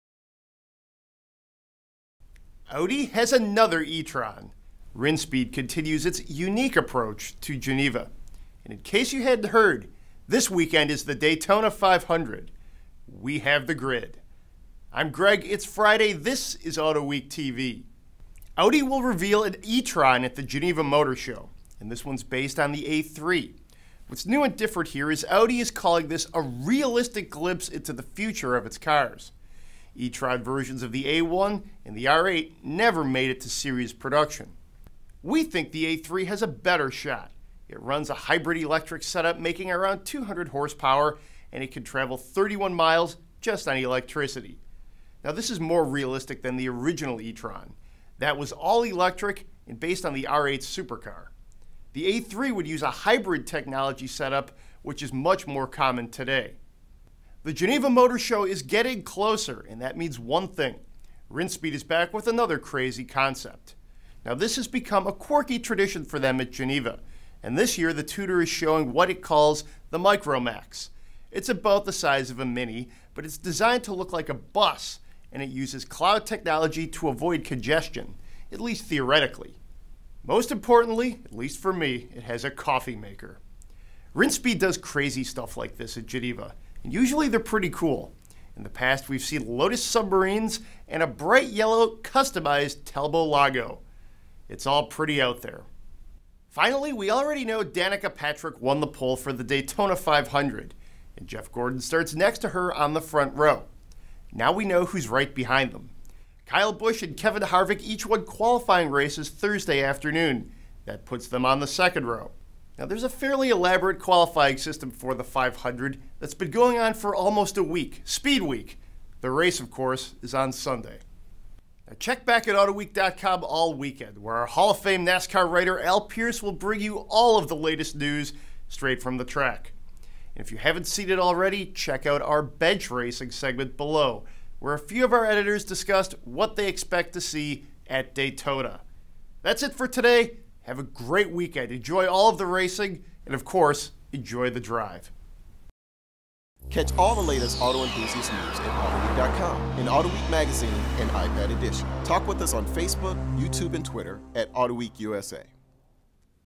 Audi E-tron to debut at Geneva motor show - Autoweek TV with Greg Migliore
Audi has another E-tron. Rinspeed has another approach for Geneva. And, in case you hadn't heard, this weekend is the Daytona 500—we've got the grid.

Audi will reveal a new E-tron at the Geneva motor show, and this one is based on the A3. What's new and different here is Audi is calling this a realistic glimpse into the future of its cars. E-tron versions of the A1 and R8 never made it to series production. We think the A3 has a better shot. It runs a hybrid-electric setup making around 200 hp, and it can travel 31 miles just on electricity. This is more realistic than the original E-tron, which was all electric and based on a supercar. The A3 would use a hybrid setup, and that technology is more common today.

The Geneva motor show is getting closer, and that means one thing: Rinspeed is back with another crazy concept. It's become a tradition of Geneva, and this year the tuner is showing the microMax. It's about the size of a Mini -- but is designed like a bus and uses cloud technology to avoid congestion. Theoretically. Most importantly, it has a coffee maker. Rinspeed does stuff like this. There always is one crazy concept on hand, and usually they're pretty cool. We're talking about Lotus submarines and bright-yellow customized Talbo-Lagos. It's all pretty out there.

And, we know Danica Patrick won the pole for the Daytona 500, and Jeff Gordon starts next to her on the front row. Now we know who's next. Kyle Busch and Kevin Harvick each won qualifying races Thursday afternoon, putting them on the second row. There's a fairly elaborate qualifying system for the 500, and the race of course, is on Sunday.

all weekend, where our hall of fame NASCAR writer Al Pearce will bring you the all of the latest news from the track.

That's it for today. Enjoy the Drive.